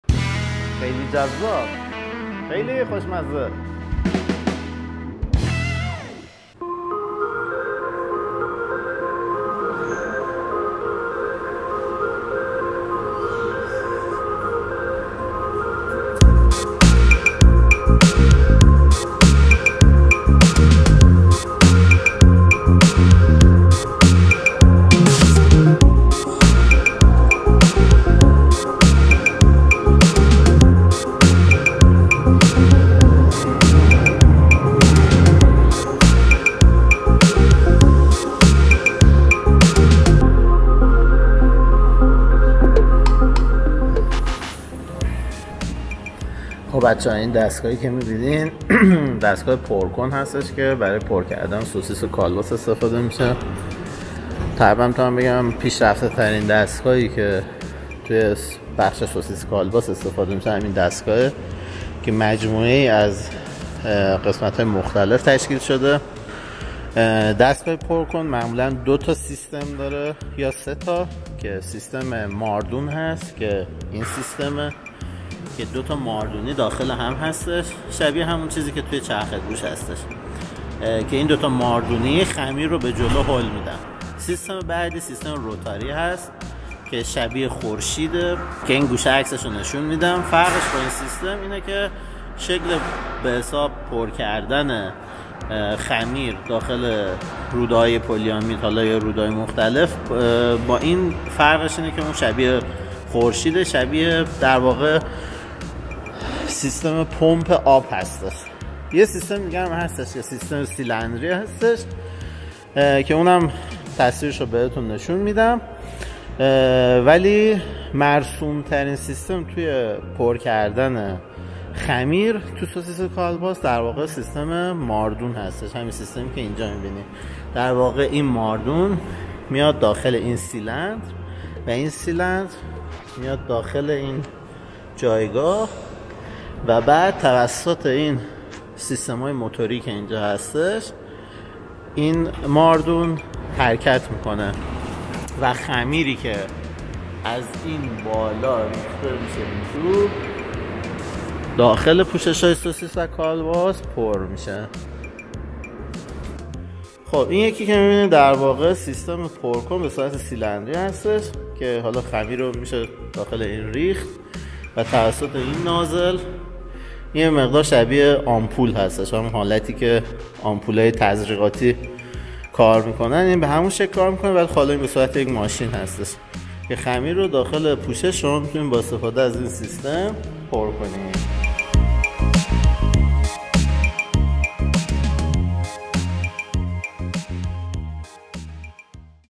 سوسیس و کالباس با چه دستگاهی داخل پوشش های پلاستیکی پر میشه
به طور کلی تو دنیای فارسی زبان اطلاعات زیادی درباره صنعت فرآورده های گوشتی وجود نداره و اگر هم اطلاعاتی باشه به صورت مقاله های پراکنده و یا در سطح رستورانی و یا خانگی هستش. برای همین توی این کانال سعی دارم بخشهایی از این صنعت رو براتون به صورت تخصصی توضیح بدم. در صنعت تولید سوسیس و کالباس ماشین آلات زیادی ممکنه استفاده بشه. ولی چند ماشین هستش که به صورت استاندارد همیشه وجود داره. اولین ماشینی که استفاده میشه چرخ گوشت هستش که برای چرخ کردن انواع گوشت و چربی مورد استفاده قرار میگیره. گوشت ها هیچ وقت به صورت درسته وارد چرخه تولید نمیشن و همیشه با پنجره های مختلف چرخ میشن. دستگاه بعدی کاتر هستش که تو ویدیو تولید سوسیس کالباس براتون نشونش دادم. این دستگاه همه چیز رو به صورت خمیر در میاره. دستگاه بعدی دستگاه پرکن هستش که توی این ویدیو میخوام براتون توضیح بدم که برای پر کردن انوان خمیر و امولسیون داخل روده های پلی آمید یا طبیعی هستش. و دستگاه آخر هم اتاق پخته که برای مرحله نهایی مورد استفاده قرار میگیره.
اگر برگردیم به دستگاه پرکن باید بگم که به طور کلی برای تولید حرفه ای سه نوع دستگاه پرکن داریم. سیستم ماردونی - سیستم روتاری و سیستم سیلندری. سیستم سیلندری که ارزانترین و شاده ترینه، برای تولیدات با مقدار کم طراحی شده و از امکانات و دقت کمی برخورداره. سیستم ماردون و روتاری سیستم های پر مصرف بازار هستند که هر کدوم مزایا و معایبی دارند. سیستم ماردونی از طول عمر بیشتر و دقت هزینه نگهداری کمتر برخورداره ولی سیستم روتاری از دقت بیشتر - عمر کمتر و هزینه نگهداری بیشتری برخورداره.